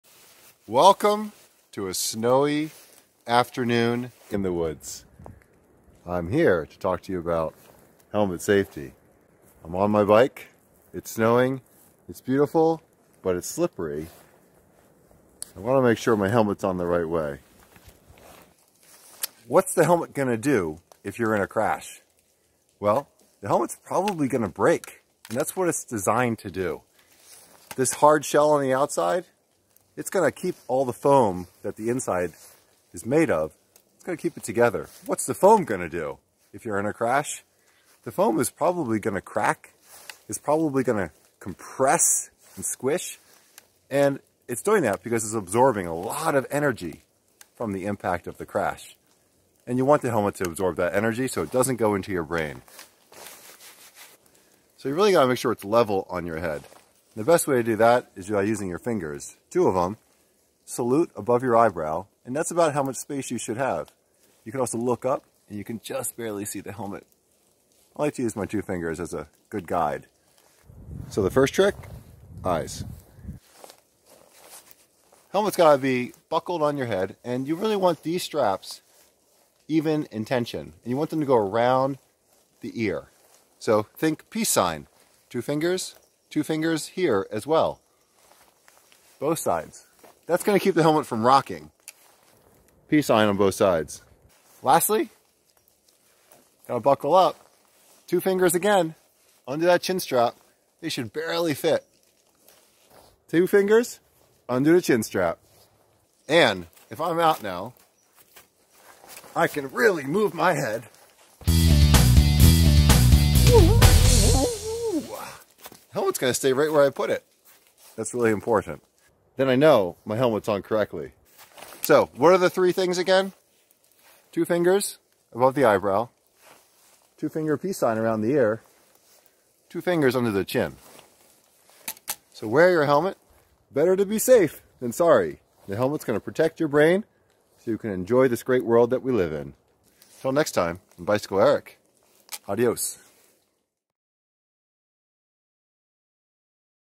1) Wear Your Helmet the Right Way
A helmet is the most important piece of protective gear you can use to shield your brain from trauma during a crash. Most bicycle crash fatalities are due to head injury; most head injuries from bike crashes would have been non-fatal had a helmet been worn.

Wear one every time you ride - and wear it properly.
First put on your helmet so it is level and snug — if it slides around, you need to insert thicker pads.
--- EYES - you should see the very edge of your helmet when you look up past your eyebrows.
--- EARS - the straps should meet right under your ear lobes to form a Y.
--- MOUTH - the strap should be loose enough so you can insert no more than two fingers between the buckle and your skin. It should be tight enough that if you drop your jaw you can feel the helmet pull down on the top of your head.

It’s the law in Maine to wear a helmet correctly if you are under 16. Do the “Eyes, Ears, Mouth Test."

Inspecting helmets: Dents and cracks mean replacement is necessary.  Helmets are good for one crash.  Helmets over 5 years old should be replaced.  Look for SNELL, ASPC, CPSC certification.